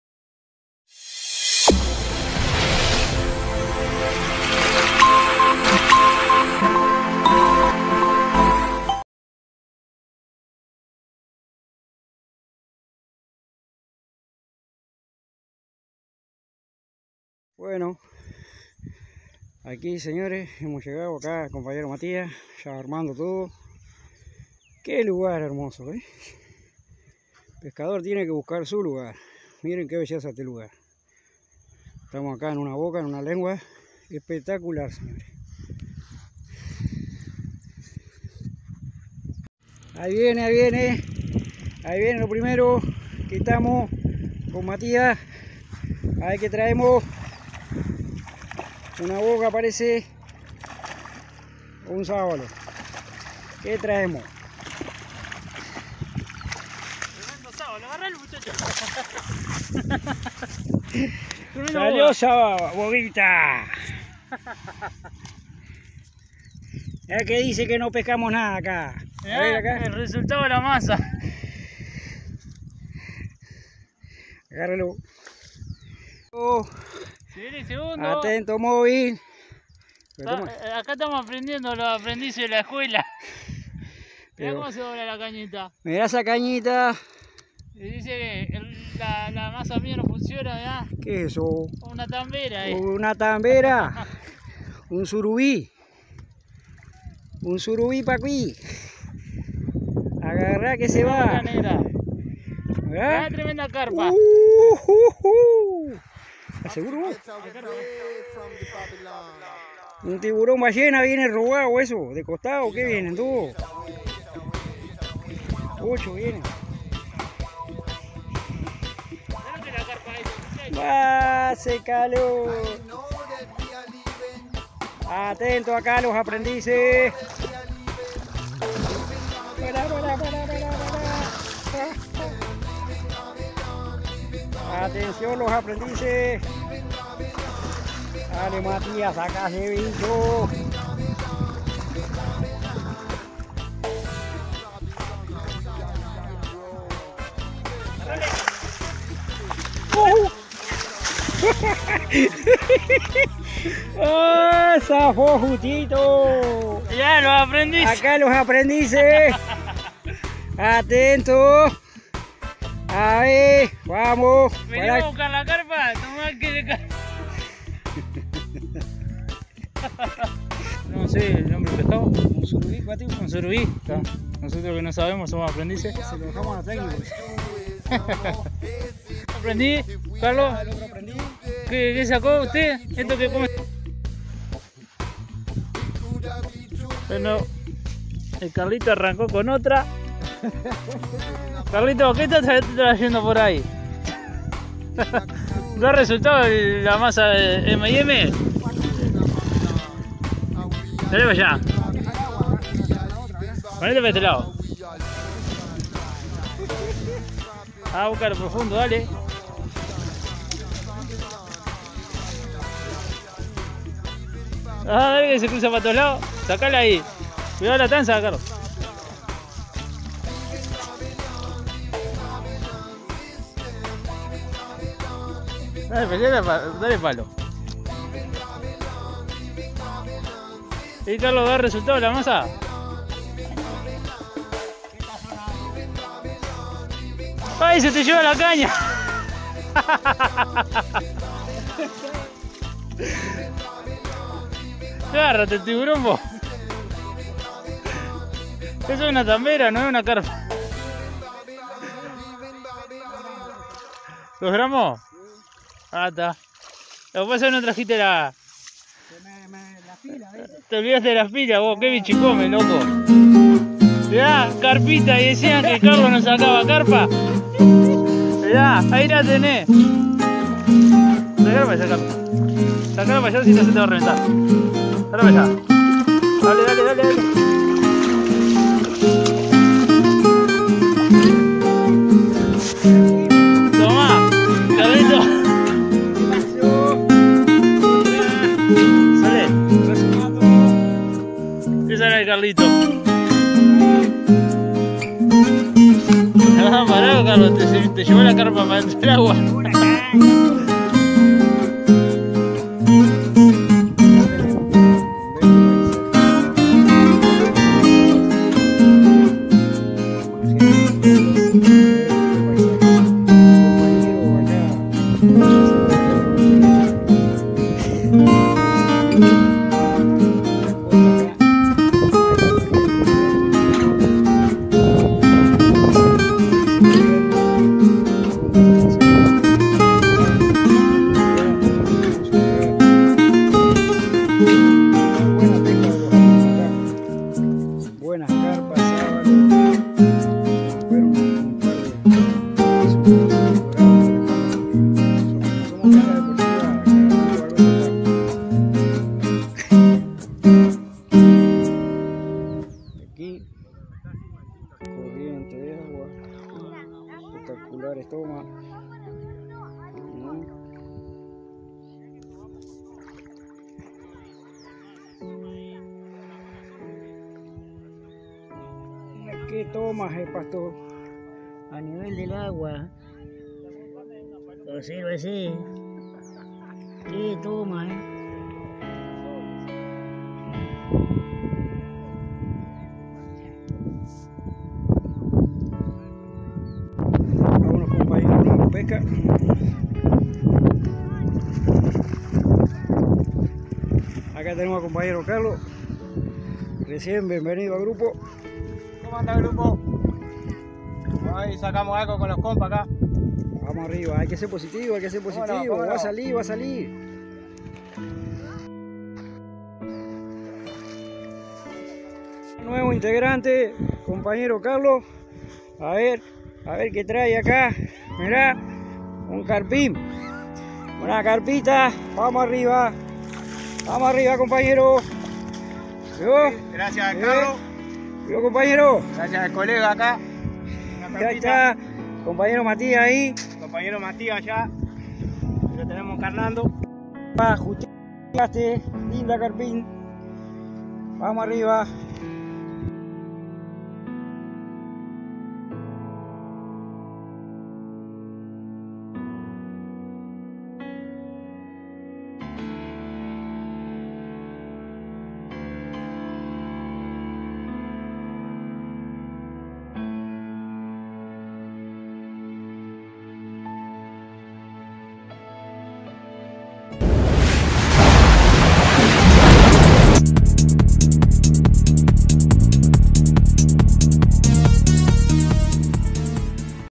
CARPAS EN AGUAS CORRIENTES , ME ARRANCO LA CAÑA DE LA MANO
LOS AMIGOS CARLOS Y MATIAS ESTE DOMINGO FUERON POR LA CARPAS , CARP FISHING EN AGUAS CORRIENTES URUGUAY Y SE LES DIO UNA PESCA ESPECTACULAR CON CAÑAS AL AGUA Y TODO.